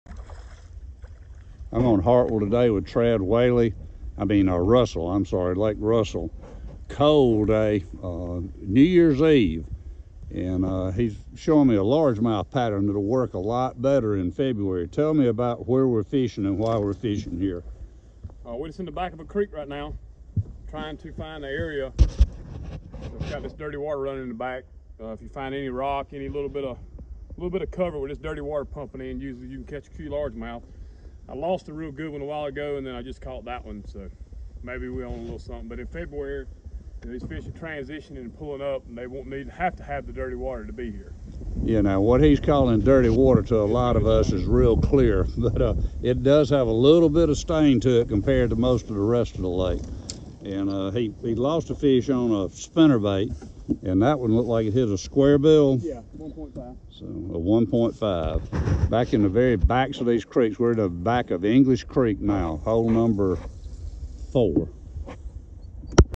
Lake Russell Bass With Trad Whaley
Trad Whaley talks about a pattern in the backs of creeks that will produce a largemouth kicker in February. It's part of the feature article Trad did with author Ronnie Garrison for the February issue of GON. Trad included several good February techniques for Russell spotted bass and largemouth, and he marked a map with 10 GPS locations to set your pattern. GON members can see the article